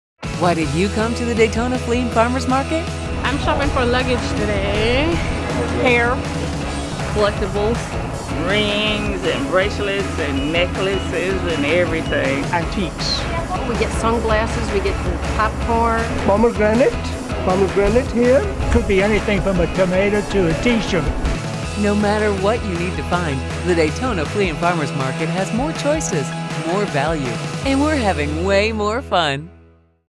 Daytona Flea & Farmer's Market in 2014 V2
Daytona Flea Market television commercial 2014